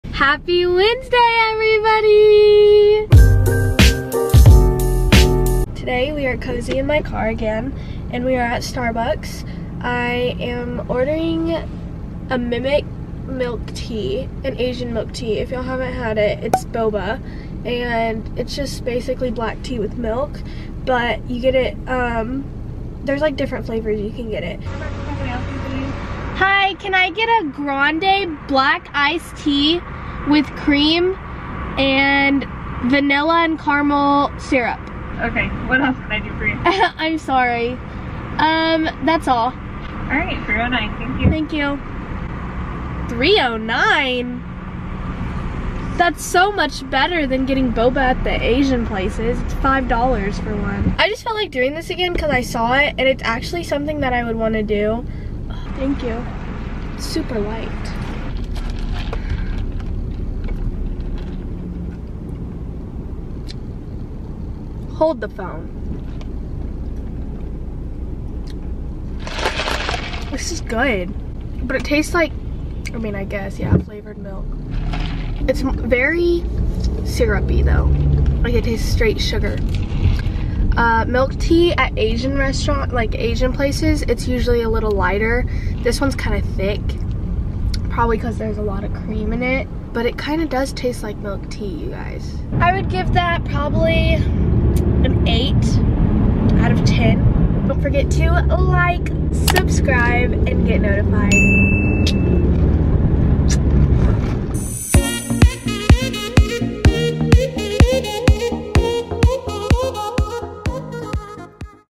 HOW TO ORDER MILK TEA AT STARBUCKS
HOW TO ORDER MILK TEA AT STARBUCKS!! I hope y'all love this video and you try it for yourself!! This week was a super short vid because there wasn't much to it but y'all seemed to like my other Starbucks one!!

All socials: Aly Immanivong